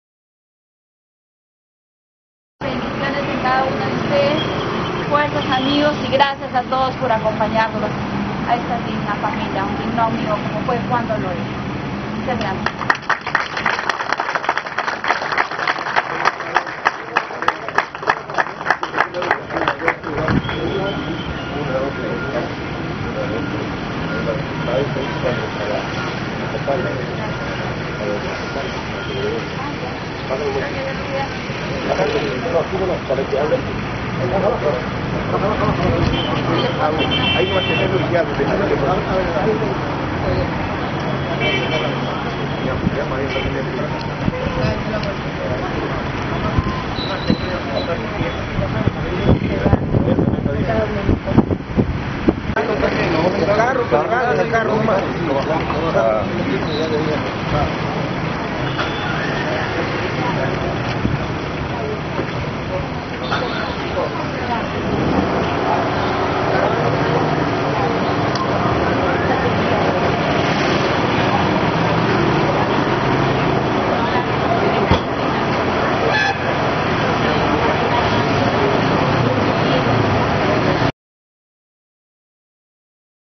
HOMENAJE PÓSTUMO EX TRABAJADOR
Alcaldesa de Castilla Violeta Ruesta de Herrera rinde un sentido homenaje póstumo al ex trabajador de la Municipalidad de Castilla, Juan Dolores Loayza, por su destacada labor edil. La burgomaestre castellana junto a todo el personal municipal manifiestan sus condolencias a la familia,esperando que el Señor lo tenga en su gloria. Don Juan descanse en paz.